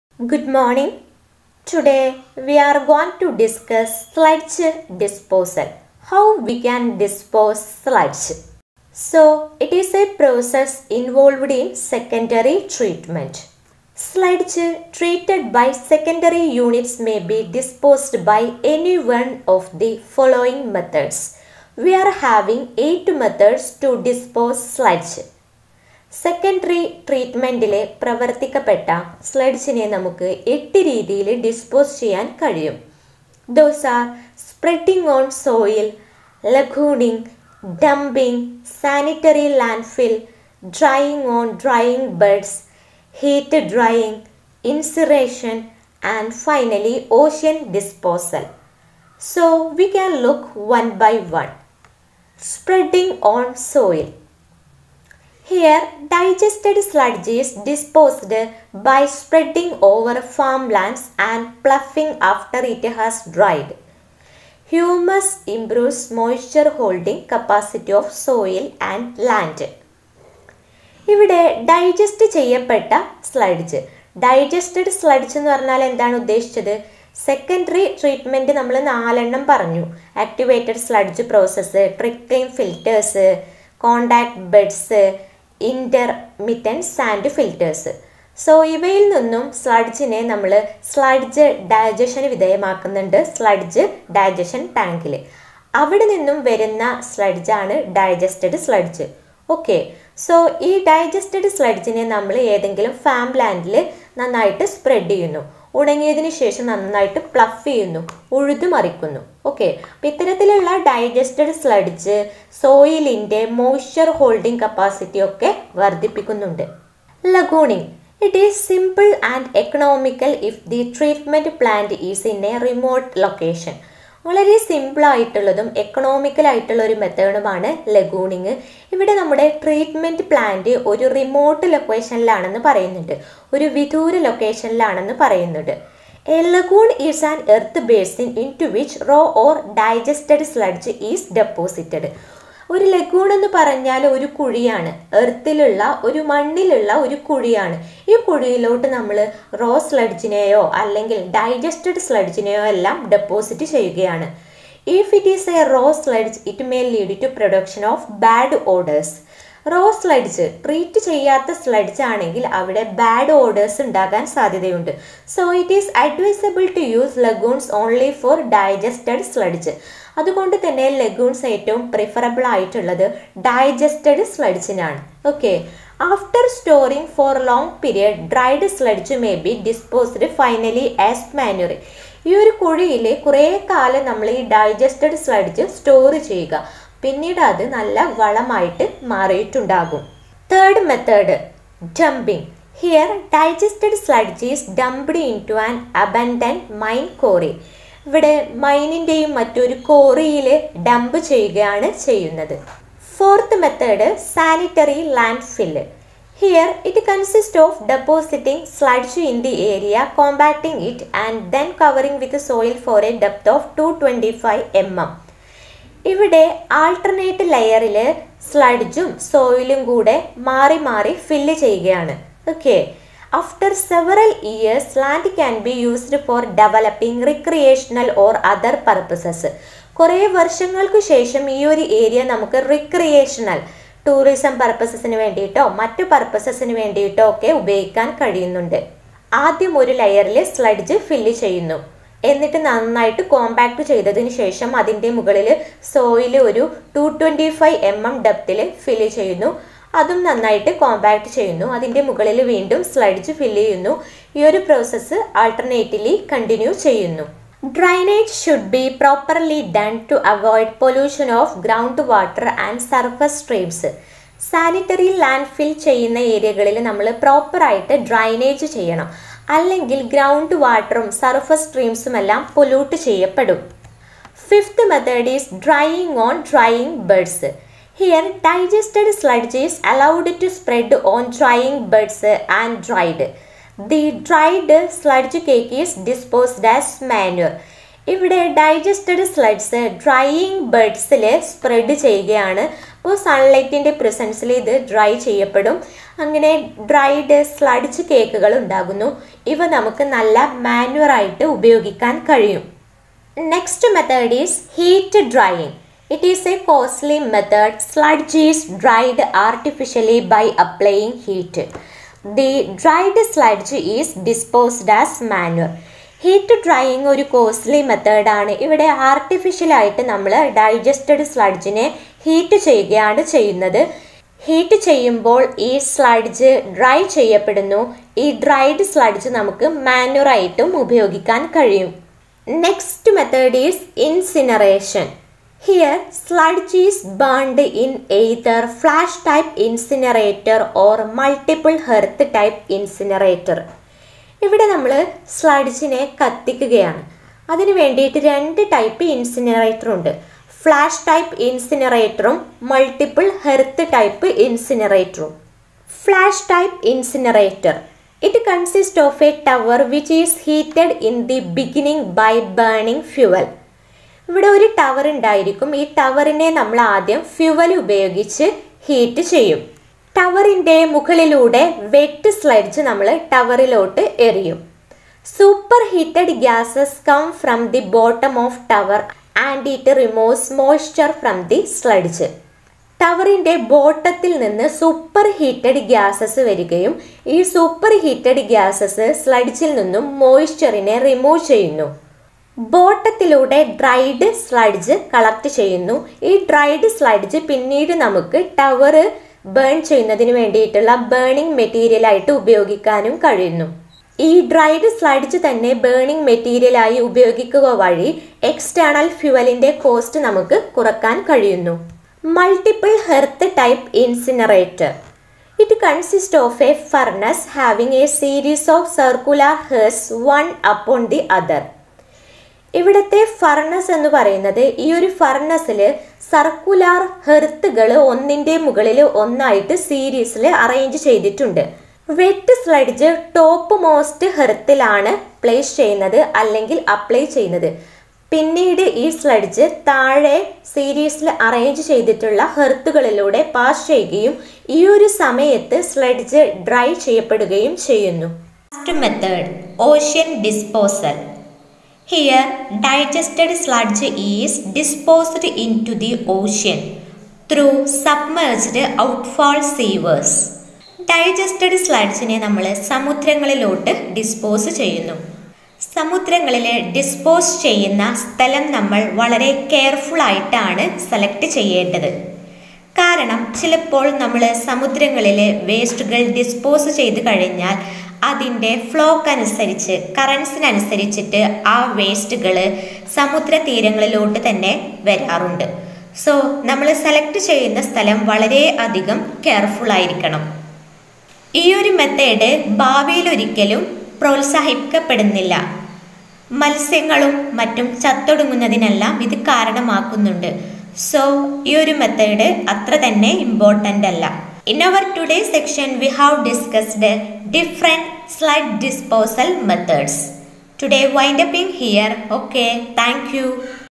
SLUDGE DISPOSAL,PART 52,ENVIRONMENTAL ENGINEERING
Sludge treated by secondary units may be disposed by any of the following methods;
1. Spreading on soil
2. Lagooning
3. Dumping
4. Sanitary land fill
5. Drying on drying beds
6. Heat drying
7. Incineration
8. Ocean disposal